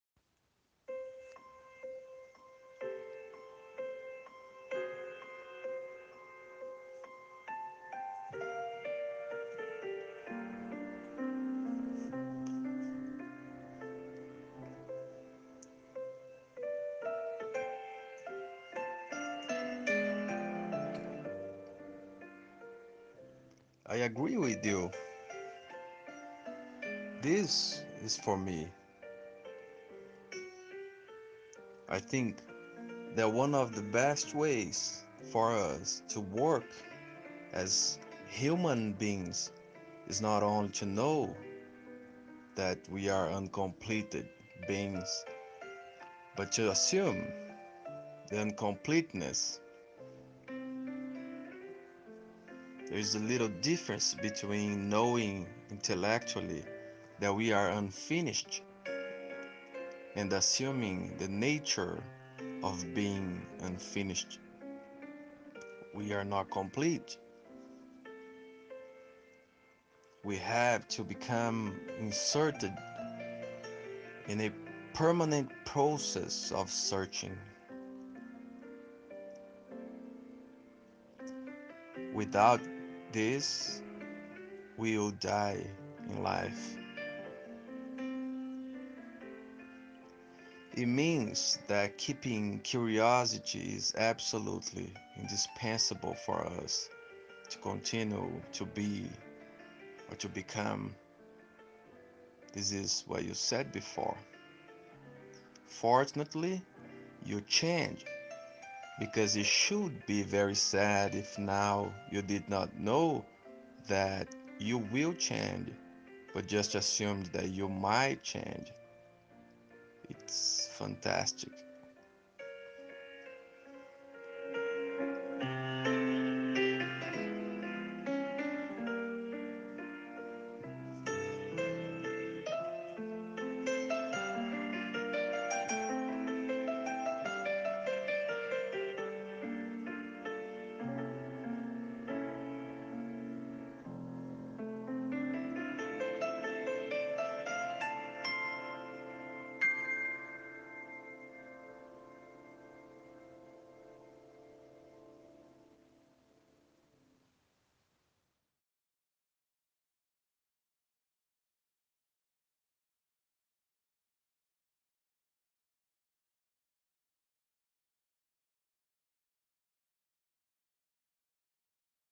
Paulo Freire: Quote from "We Make the Road by Walking"
Book: We Make the Road by Walking: Conversations on Education and Social Change by Paulo Freire and Myles Horton